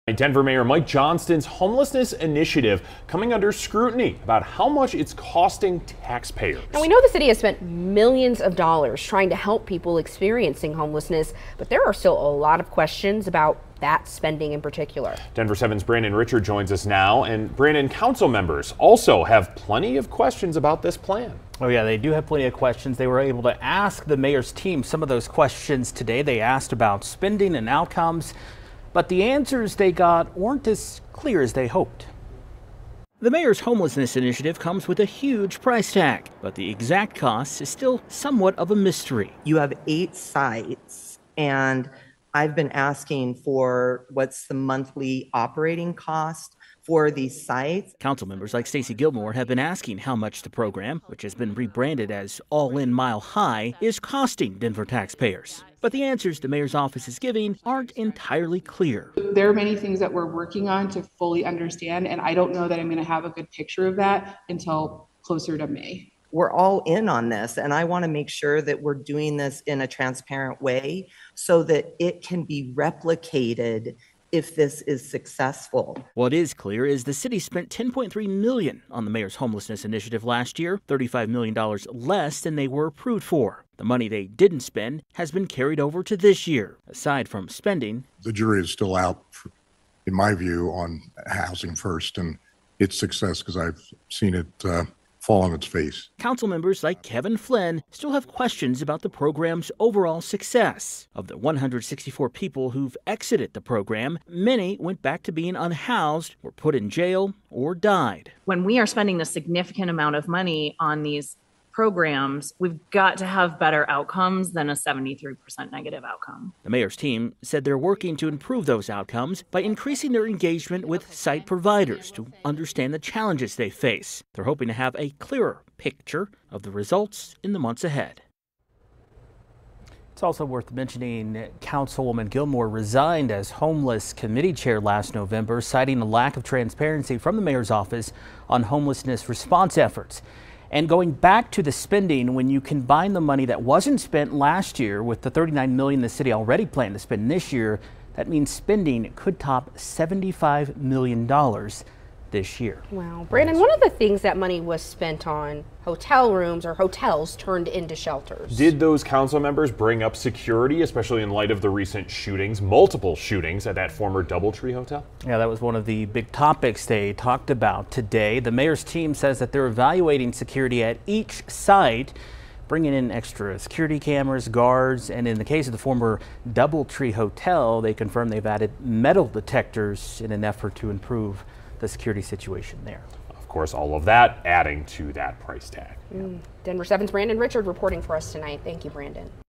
How much is Mayor Johnston’s plan to address homelessness costing Denver taxpayers?
The Denver City Council is pressing the mayor’s office on just how much the city is spending and the cost to taxpayers as Johnston addresses homelessness in Denver.